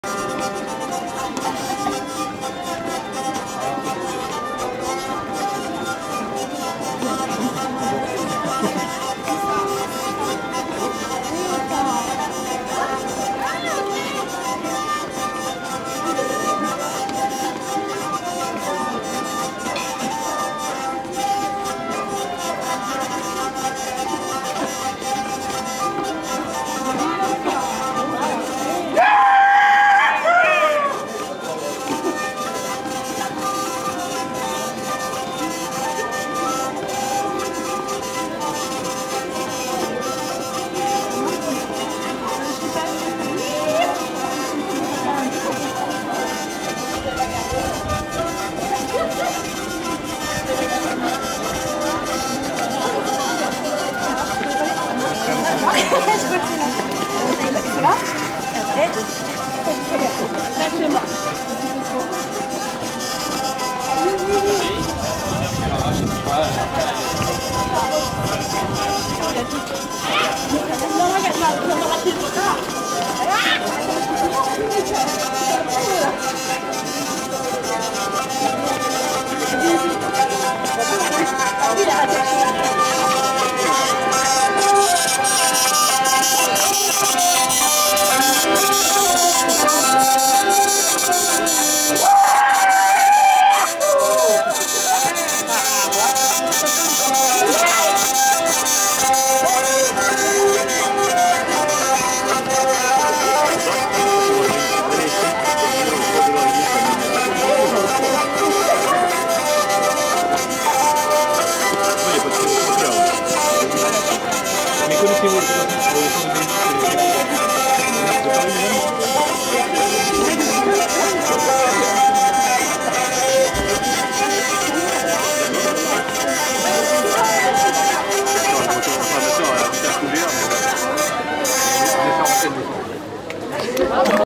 Danser Encore la Maraîchine
Cette Maraîchine jouée par Richard au Bal Folk Gratuit et Participatif Organisé par Folk en Seine dans le Parc de   Belleville 75020 à Paris  .